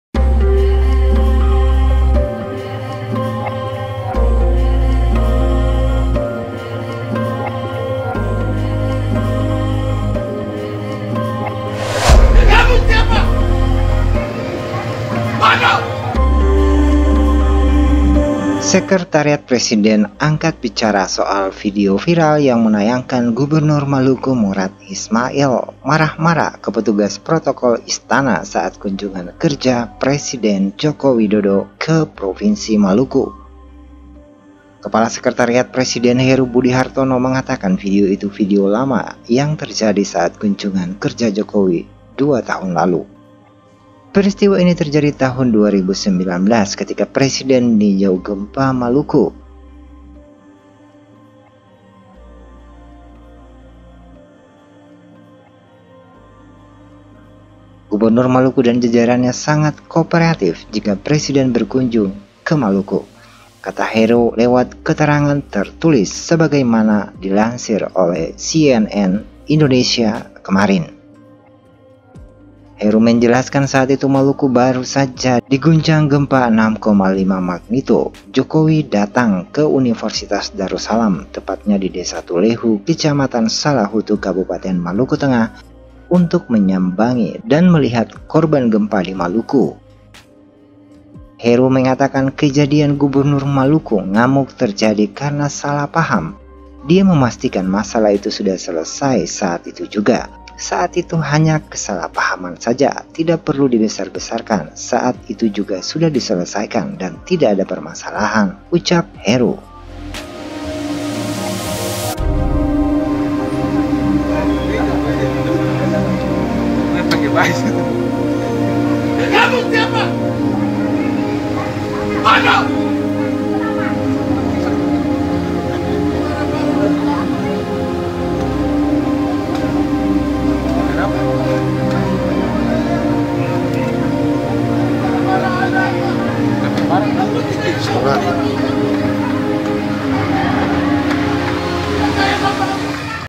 Jawaban Istana Soal Video Viral Gubernur Maluku
Kepala Sekretariat Presiden Heru Budi Hartono akhirnya angkat bicara soal video viral yang menayangkan Gubernur Maluku Murad Ismail marah-marah ke petugas protokoler Istana saat kunjungan kerja Presiden Joko Widodo. seperti apa pendapat istana soal video Peristiwa ini terjadi tahun 2019 ketika Presiden meninjau gempa di Maluku itu ?

- Ruang Publik
- Informasi Baru
- Info Menarik
- Sisi Lain Dari Berita
- Berita Terkini
- Human Interes